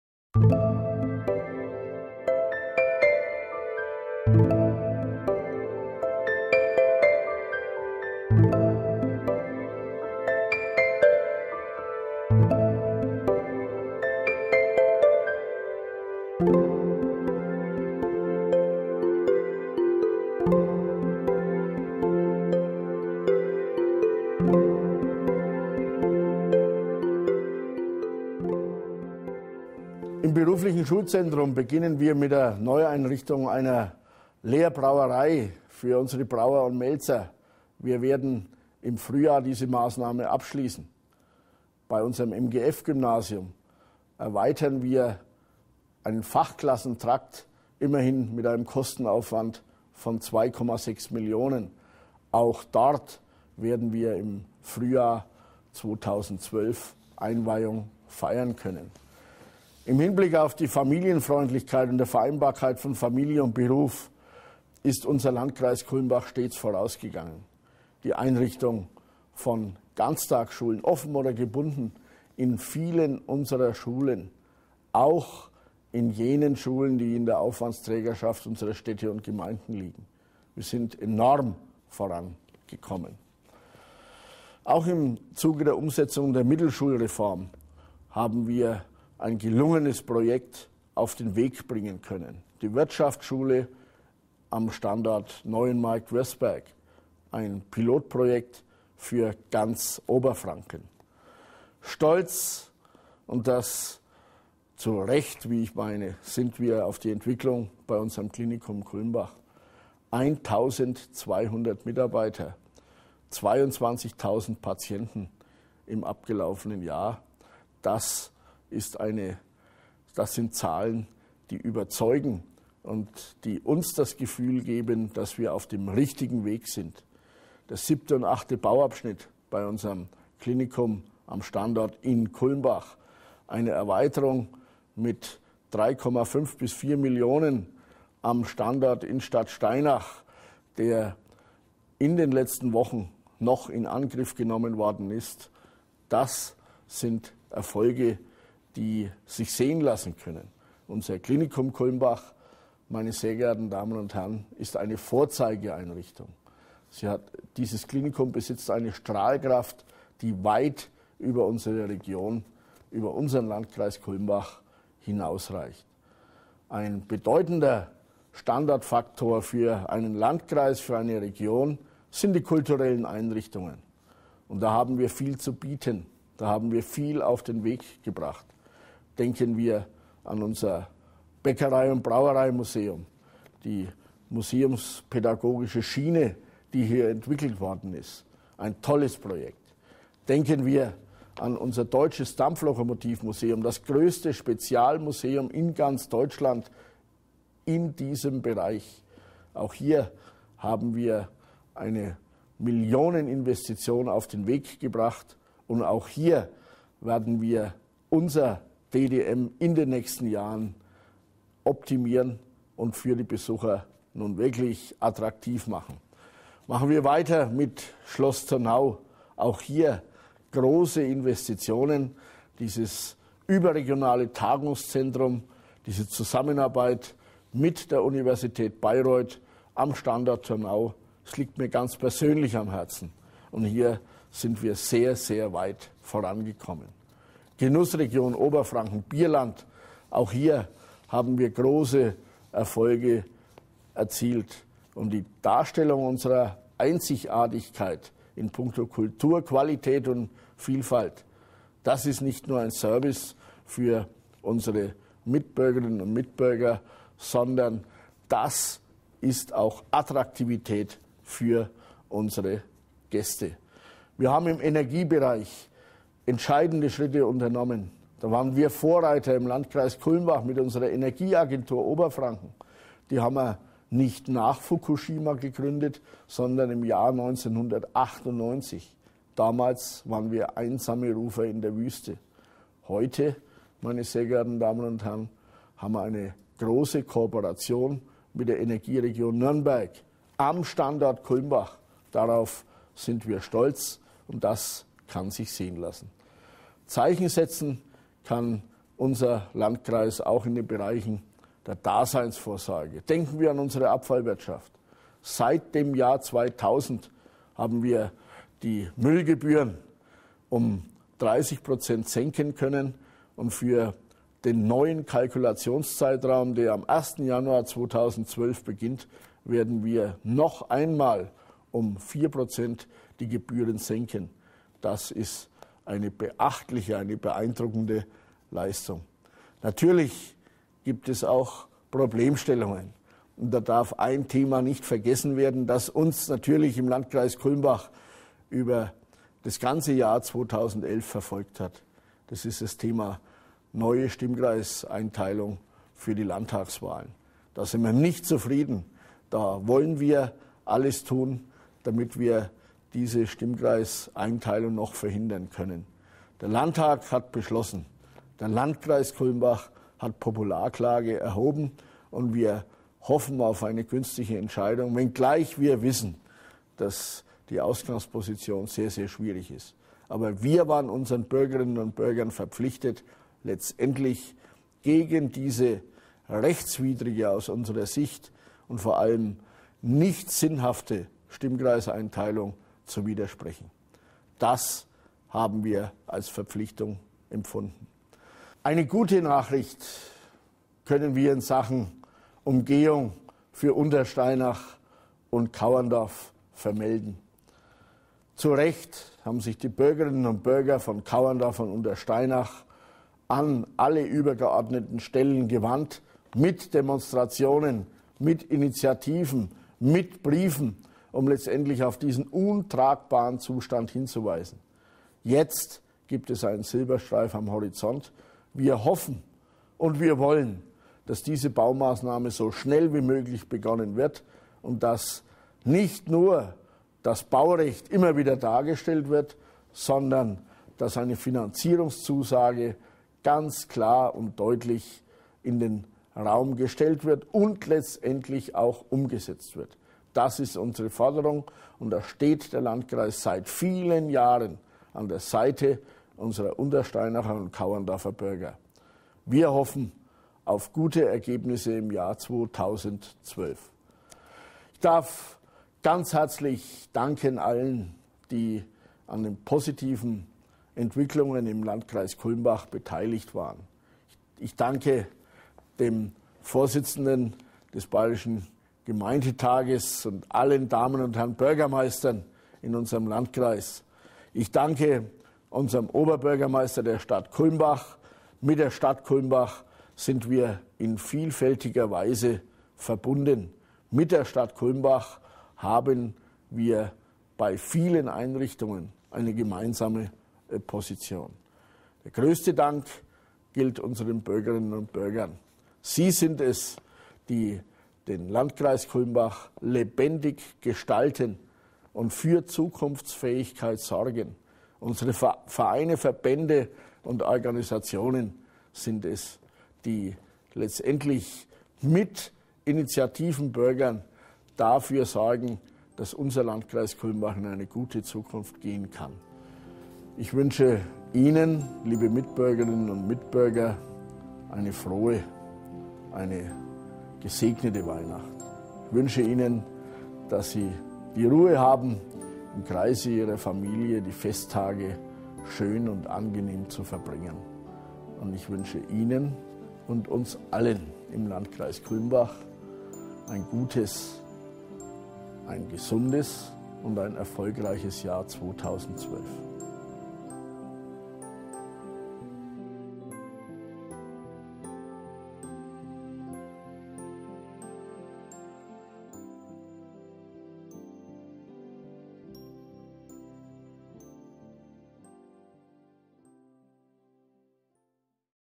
Teil 2 der Jahresbilanz mit Klaus Peter Söllner
Trotz wirtschaftlich schwieriger Zeiten steht der Landkreis Kulmbach noch immer gut da. Folgen Sie in Teil 2 der Jahresbilanz des Kulmbacher Landrats den Spuren die das vergangene Jahr in den Gemeinden hinterlassen haben.

Redaktion: Céline Koch
Kameras: Martin Lauterbach
Licht & Ton: Michael Schoberth
Assistenz: Eva Maria Thoma
Schnitt: Karl-Ernst Reichel
Ausführender Produzent: Axel Klawuhn